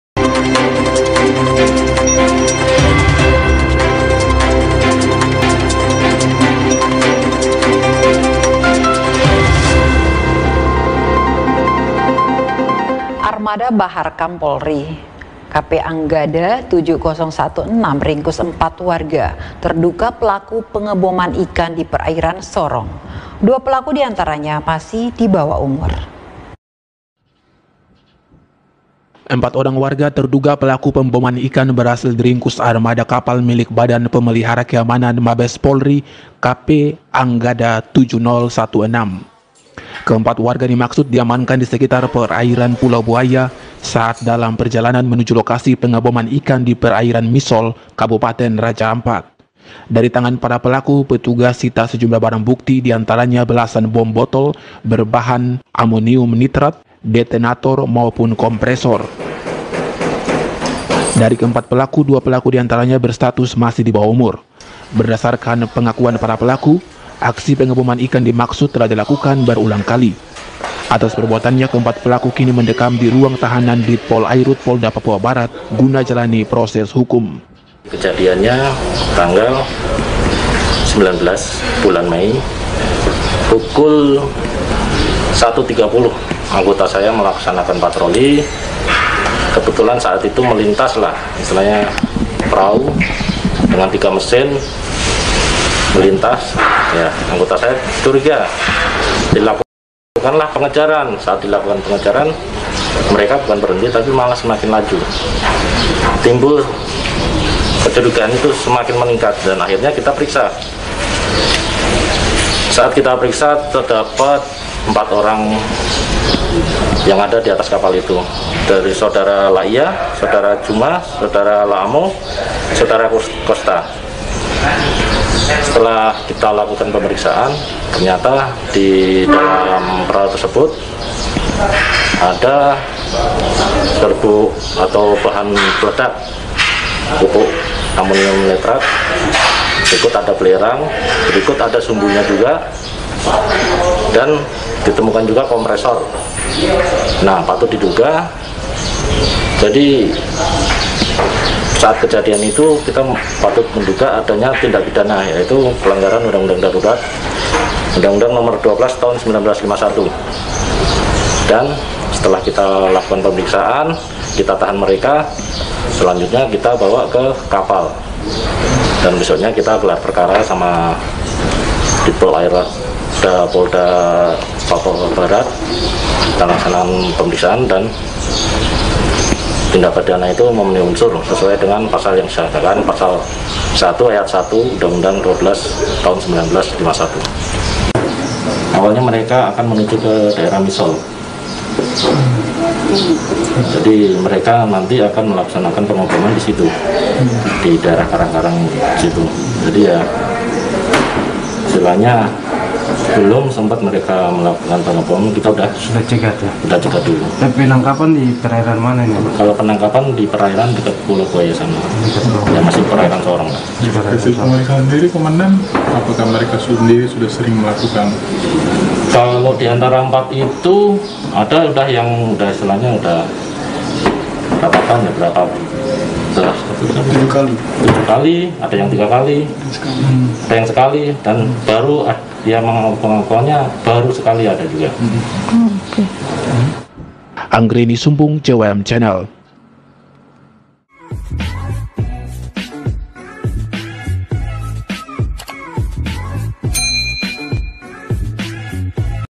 MABES POLRI - Armada Baharkam Polri Ringkus Pelaku Pengeboman Ikan.@CWMNEWS
MABES POLRI - Armada Baharkam Polri Ringkus Pelaku Pengeboman Ikan.